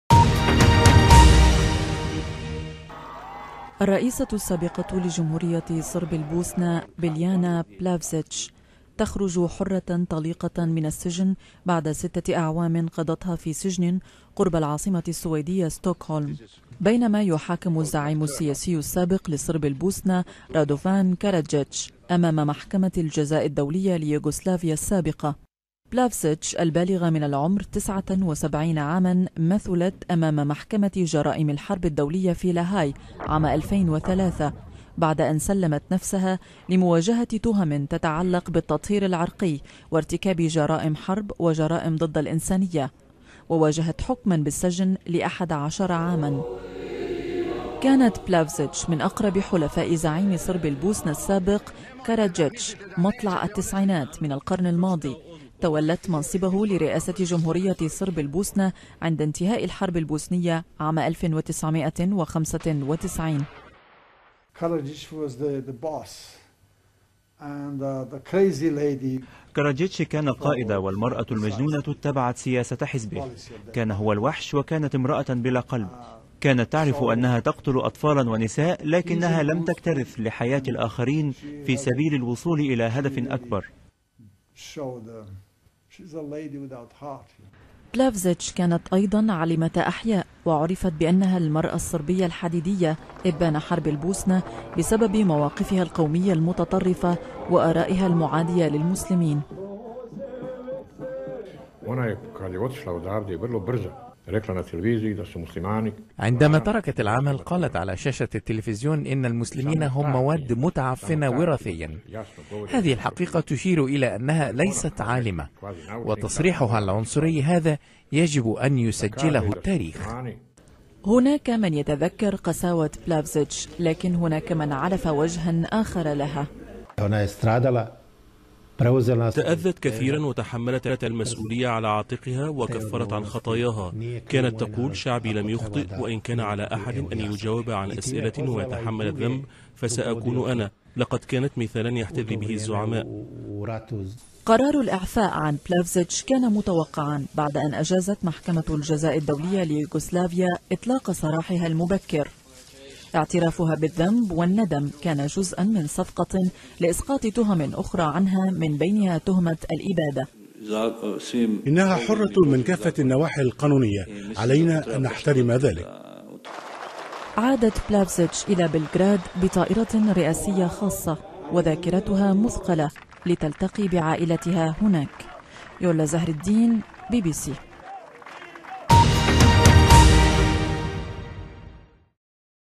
استئناف محاكمة كراجيتش في لاهاي اليوم
تستأنف اليوم الثلاثاء محاكمة زعيم صرب البوسنة السابق رادوفان كراجيتش امام المحكمة الدولية لجرائم الحرب في يوغسلافيا السابقة في لاهاي.